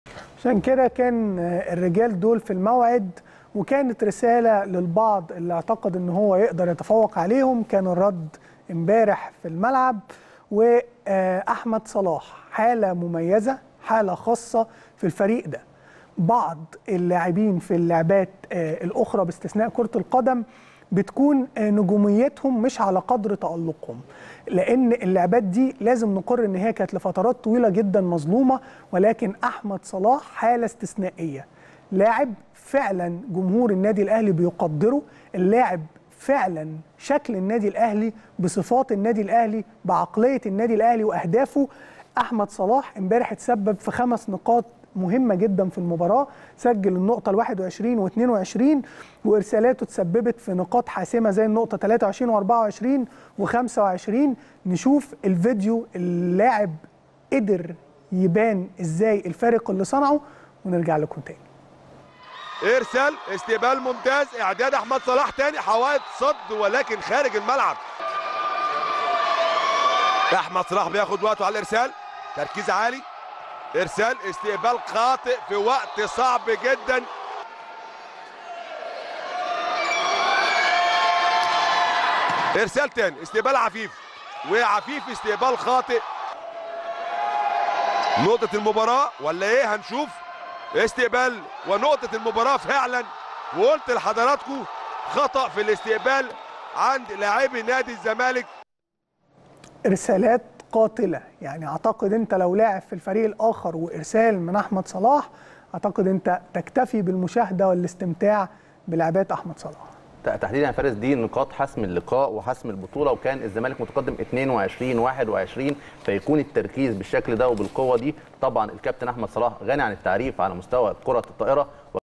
"أحمد صلاح".. حالة إستثنائية وارسالات حاسمة في فوز الماسترز على الزمالك في نهائي البطولة العربية
التردد: 12341 أفقي.
القمر: نايل سات.
معامل الخطأ: 5/6.
معدل الترميز: 27500